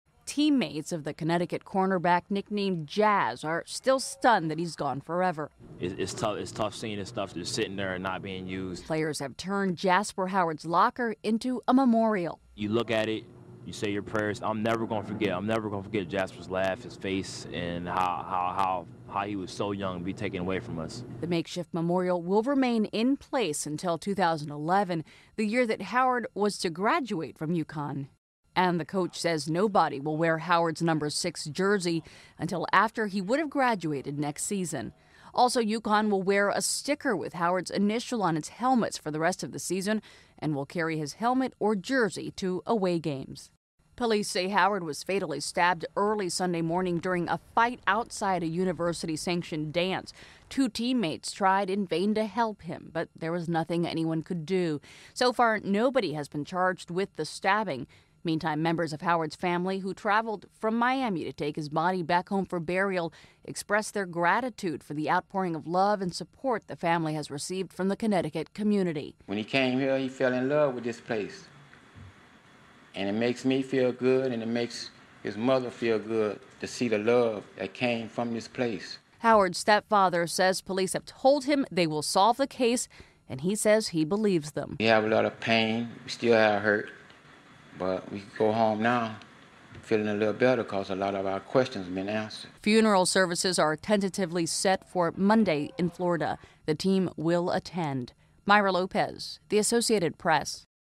No Arrest Yet in Stabbing of UConn Player
So far, no arrests have been made in the stabbing death of a University of Connecticut player. However, his family will head home to Miami, Florida soon, taking their son Jasper Howard back for burial.  (Oct. 21)